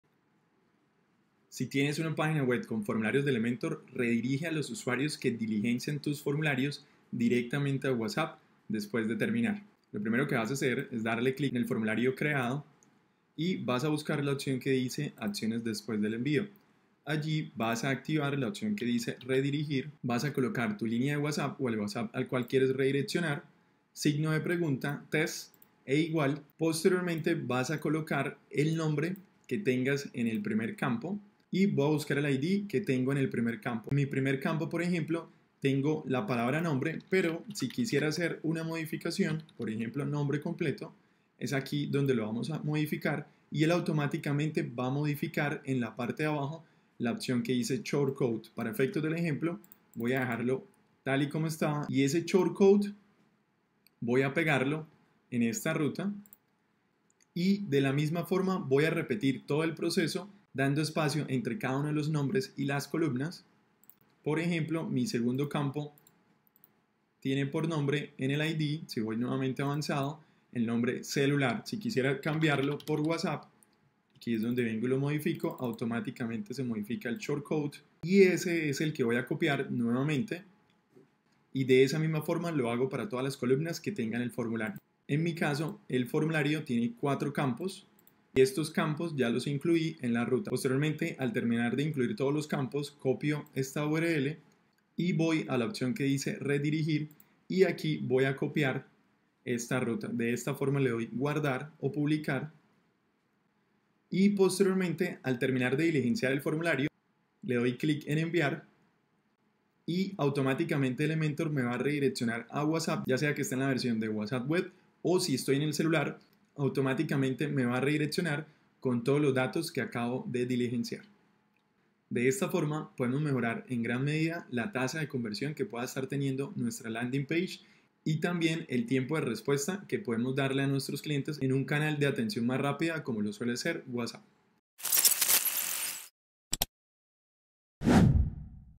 👉Redirige formularios de Elementor Pro a WhatsApp (sin plugins)
Redirige tus formularios de Elementor Pro con WhatsApp (sin plugins)

¿Quieres que los mensajes de tu formulario en Elementor Pro lleguen directamente a WhatsApp?
En este tutorial te explico paso a paso cómo hacerlo sin instalar plugins extra.
Aprende a conectar WhatsApp con tu formulario de Elementor y mejora la conversión de tus leads
✅ Sin plugins
✅ 100% Elementor Pro
✅ Rápido, directo y funcional

*reemplaza por tu número (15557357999) de whatsapp

*incluye (  %20  )  para generar espacios entre campos